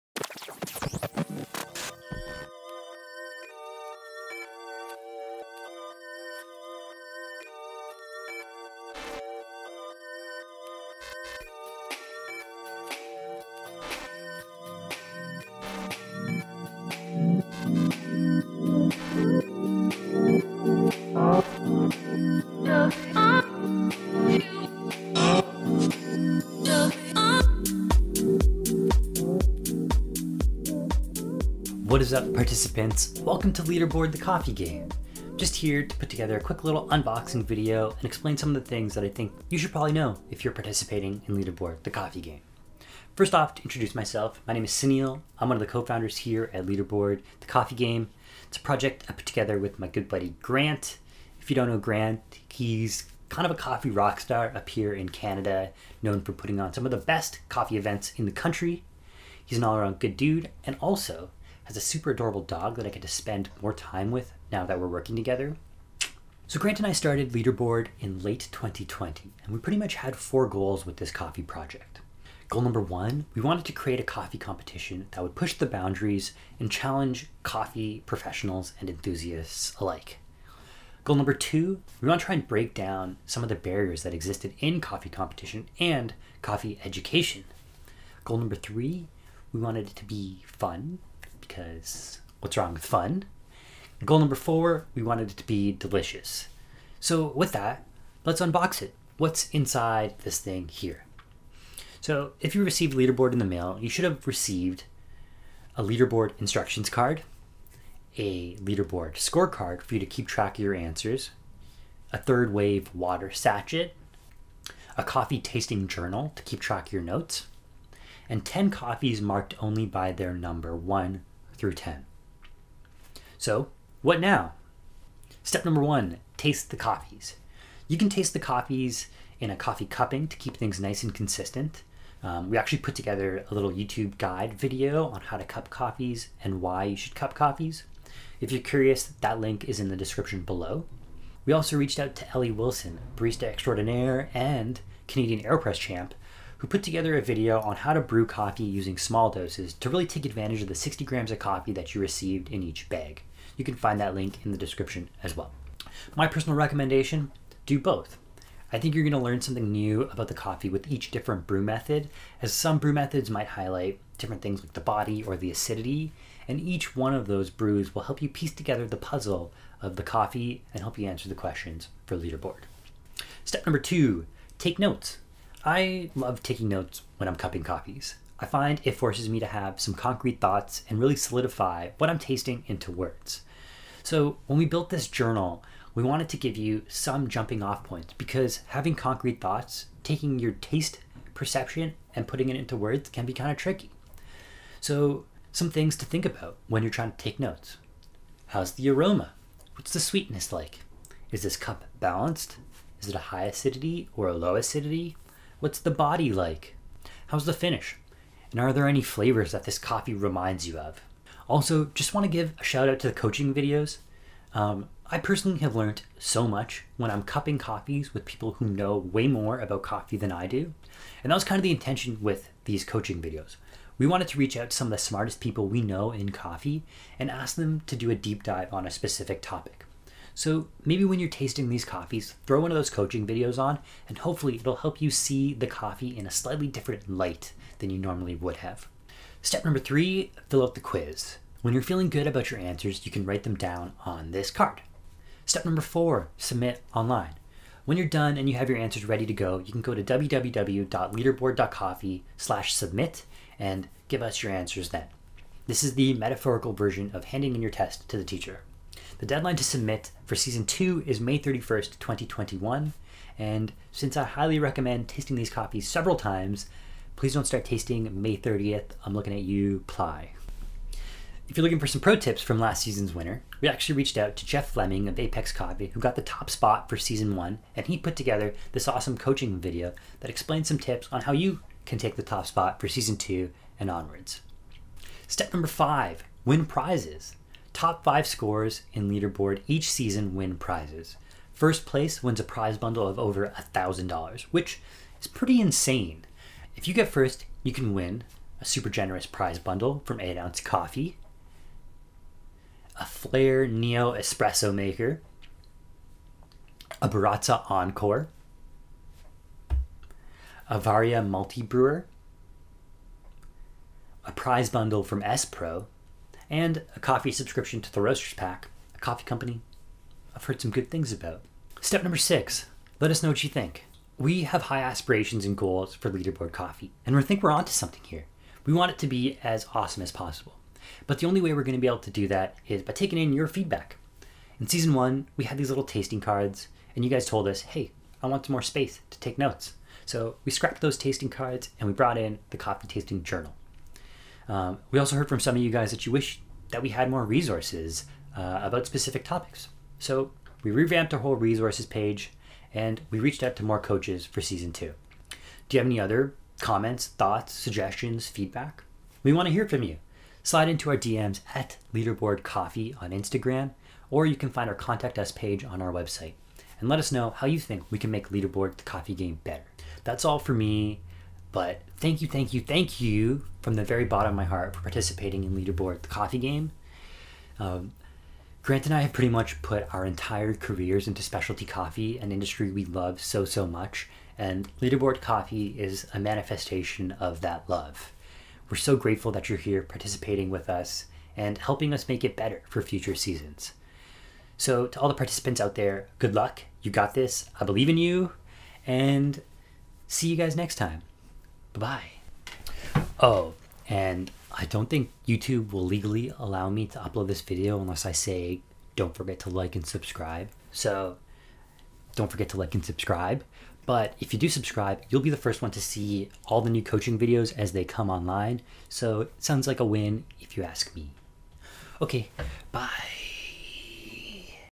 Unboxing Leaderboard: The Coffee Game
Leaderboard is a quarterly coffee tasting event and competition where players receive 10 mystery coffees from 10 roasters and compete to identify them. Play with friends virtually and learn from coffee professionals around the world to win thousands in prizes.